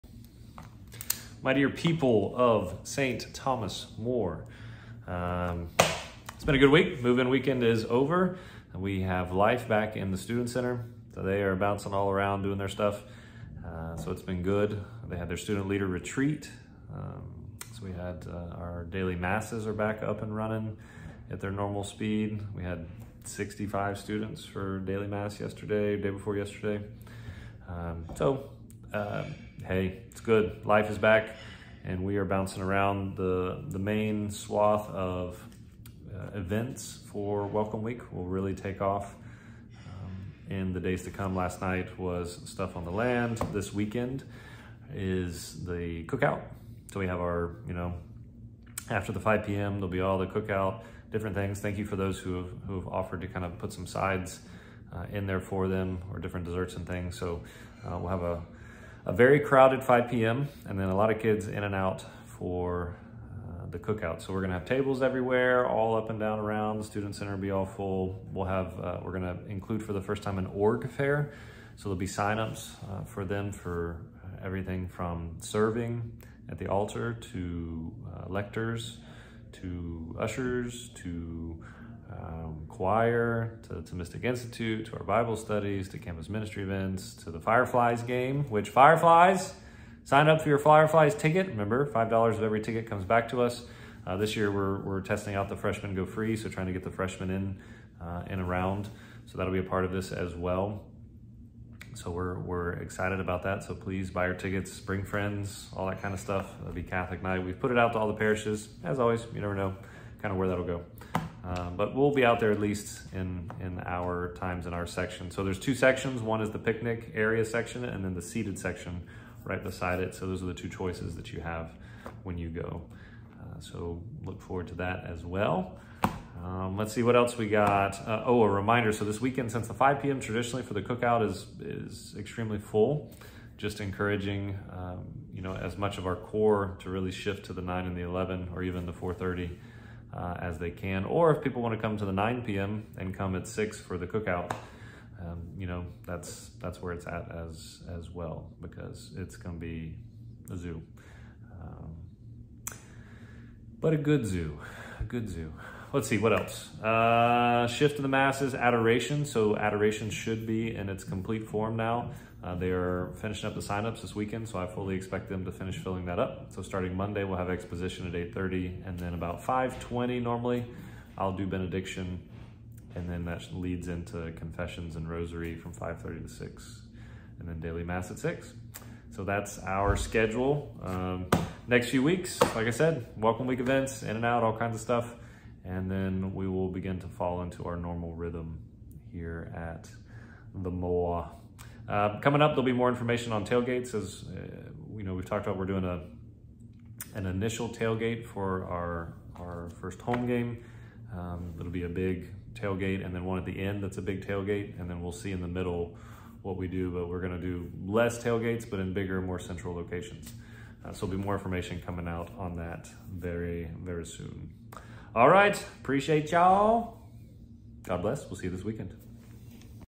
STM Weekend Update, 8.26.23
Student Leadership Team retreat; daily Masses are back; Welcome Week events coming up - welcome back cookout on Sunday after the 5pm Mass; STM Org Fair; Fireflies game on Tuesday; all day Adoration starts Monday.